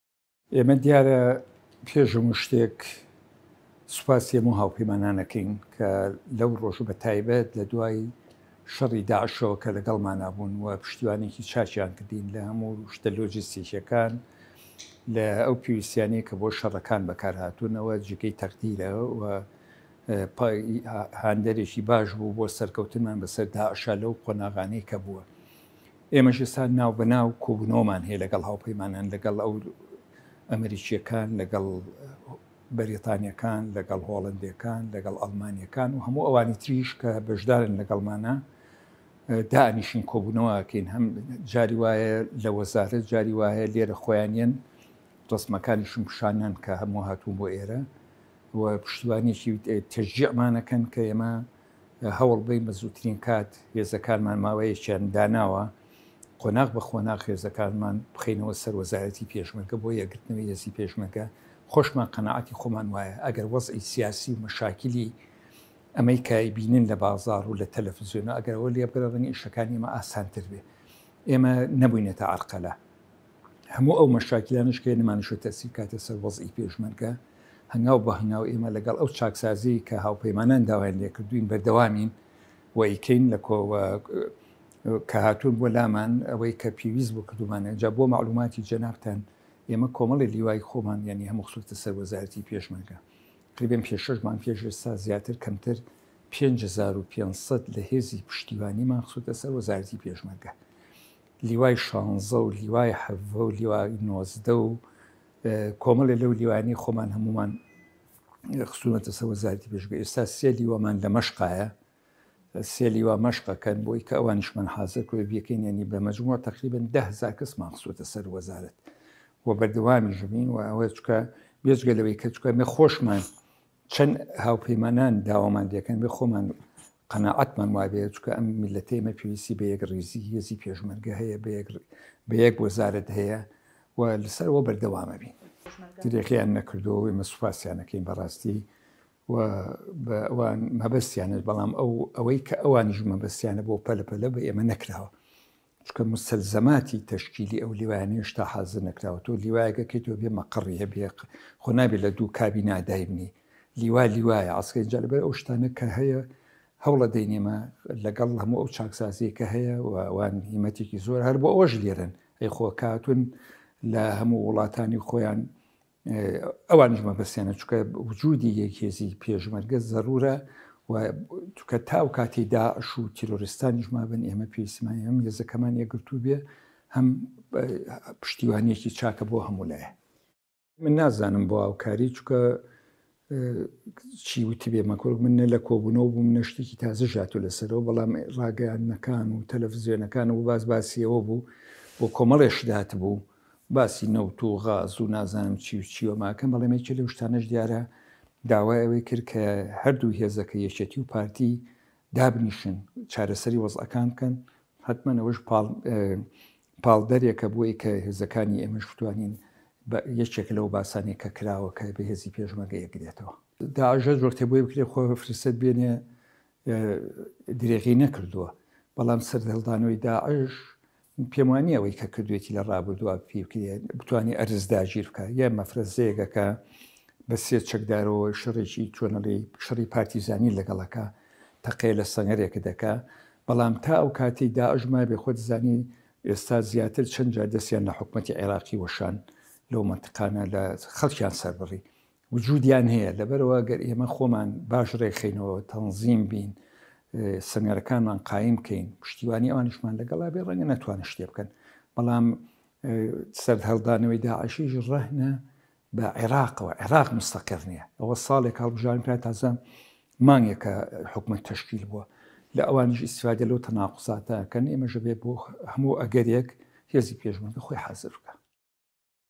دەنگی ئەمەریکا VOA | لێدوانی مستەفا چاوڕەش فەرماندەی گشتی هێزەکانی حەفتا
مستەفا چاوڕەش فەرماندەی گشتی هێزەکانی حەفتا باس لە هەوڵەکانی هێزەکانی هاوپەیمانان بەسەرکردایەتی ئەمەریکا دەکات لەسەر یەکگرتنەوەی یەکەی حەفتا و هەشتای هێزەکانی پێشمەرگەی هەرێمی کوردستان.